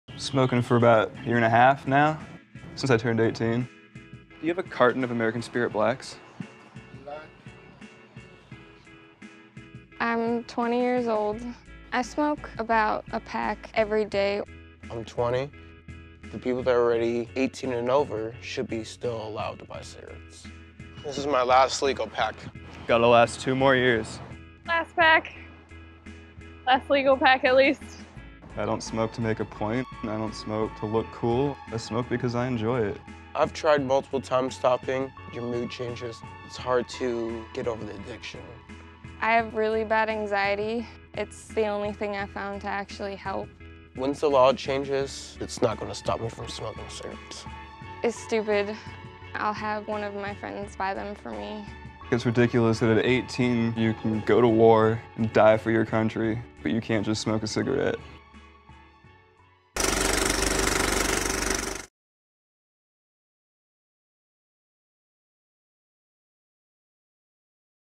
California's Teen Smokers // 60 Second Docs
California's new law raising the age at which one can legally purchase cigarettes to 21 went into effect on June 9, 2016. Teen smokers around the state are now trying to kick the habit -- or looking for more unsavory ways to feed their habit. Here are three youngsters caught in the throes of this struggle.

60 Second Docs